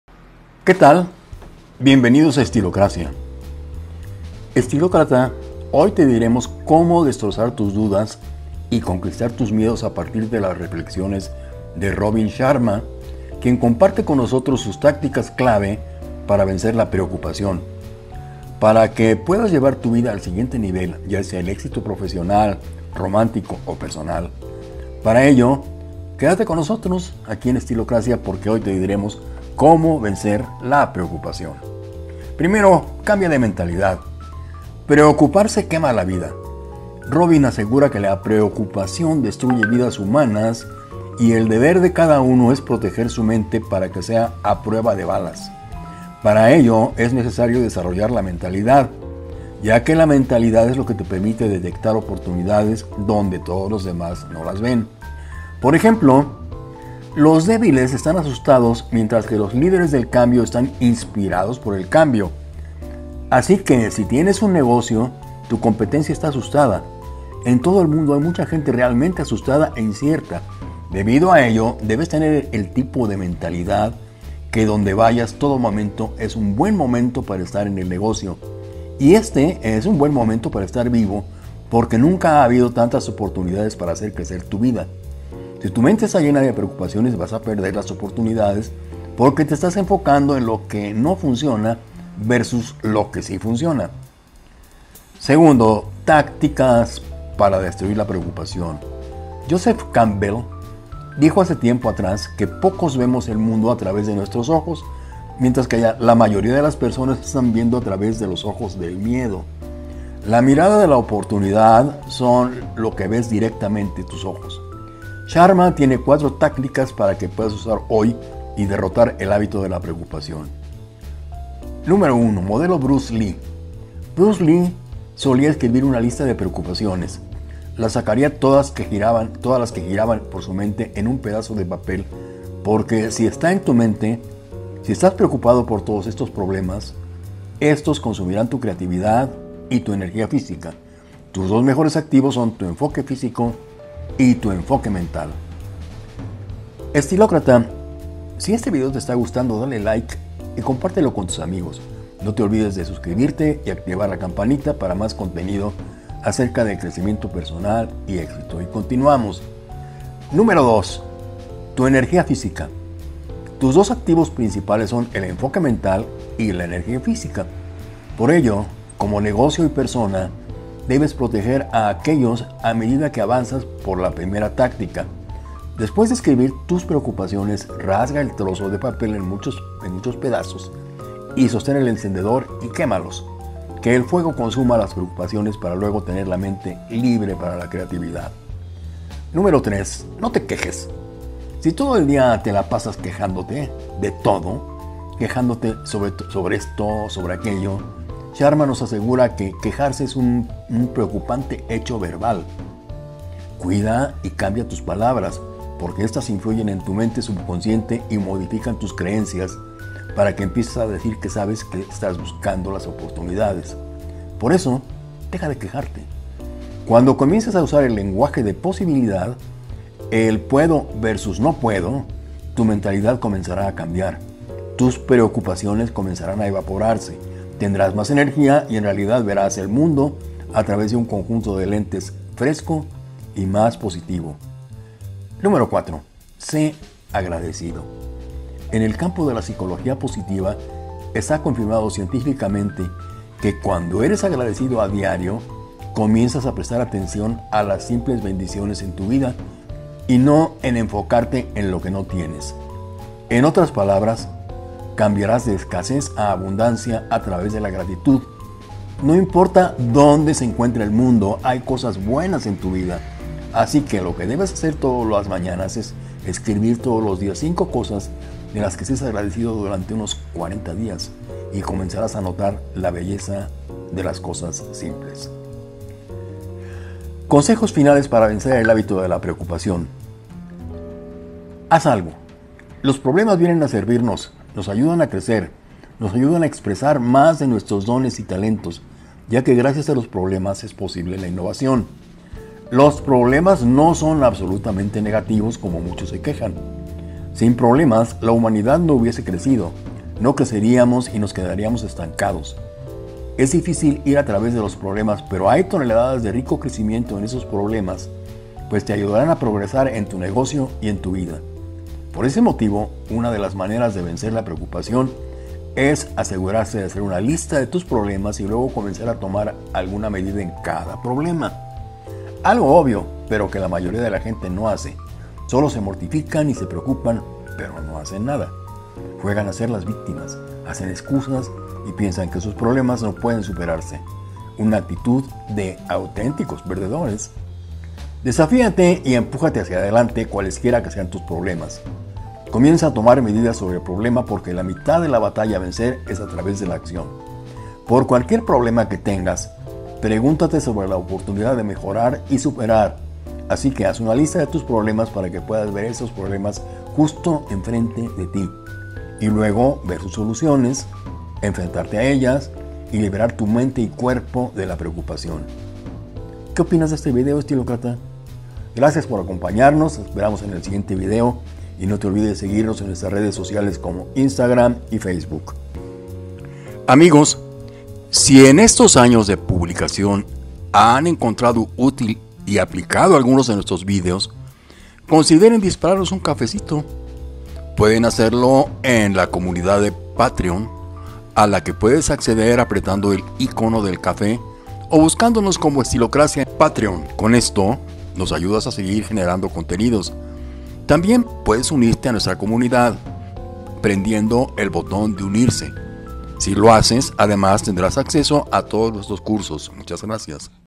CÓMO VENCER LAS PREOCUPACIONES? VÉNCELAS CON ESTOS SENCILLOS PASOS
DEJA DE PREOCUPARTE Y VAS A CAMBIAR TU VIDA DE INMEDIAT, YO SÉ LO QUE TE DIGO.

Visita nuestro sitio asociado de Amazon para productos de tu interés: